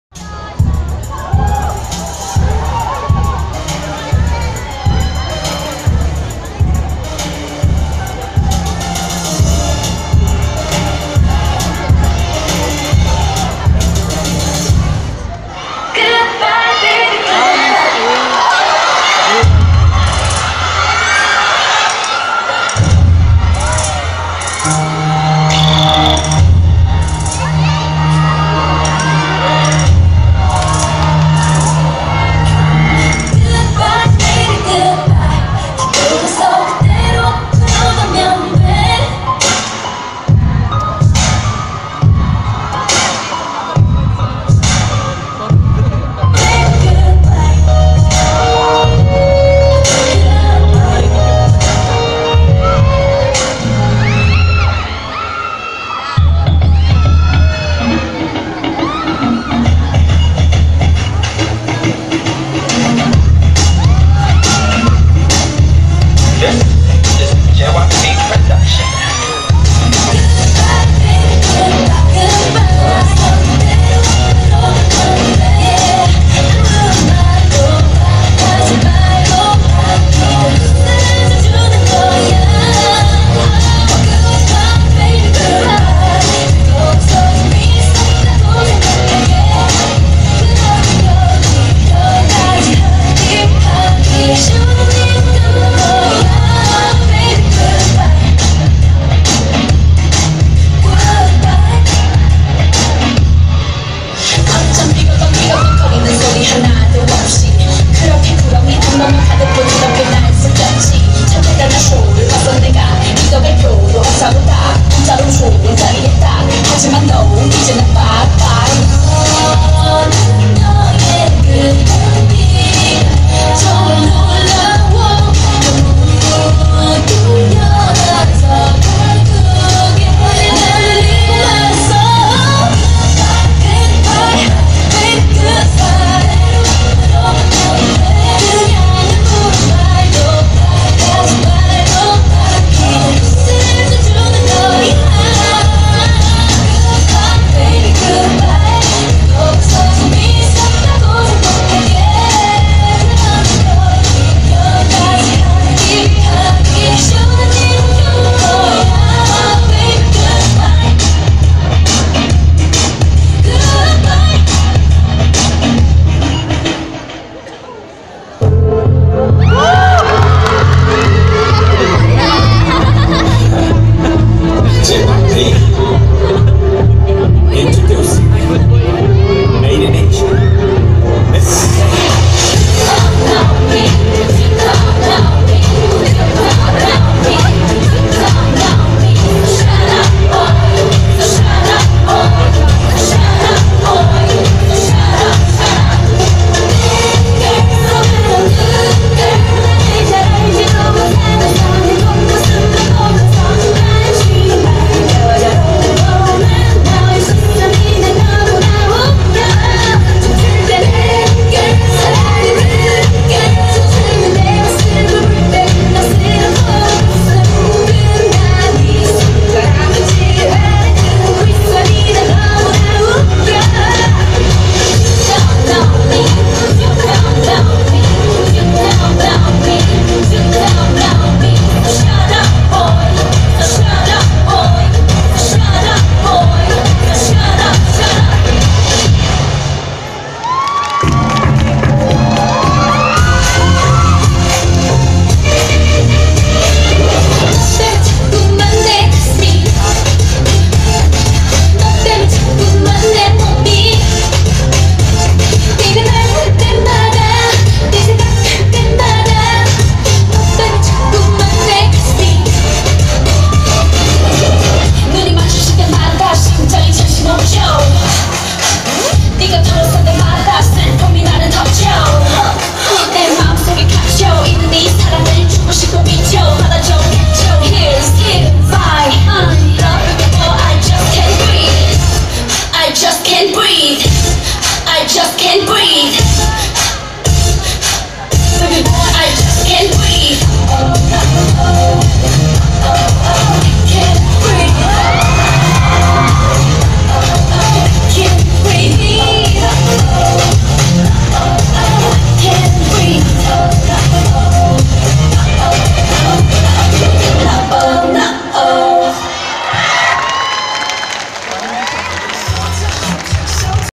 Bottoms - Miss A KPTF Kpop Contest
what's up guys we are bottoms covering Miss A . were joining in last korean trade fair kpop contest so this is our performance video .
ㅁㅁㅁㅁㅁㅁㅁㅁㅁㅁㅁㅁㅁㅁㅁ

Be yourself and glow any time . Only love no hate ❤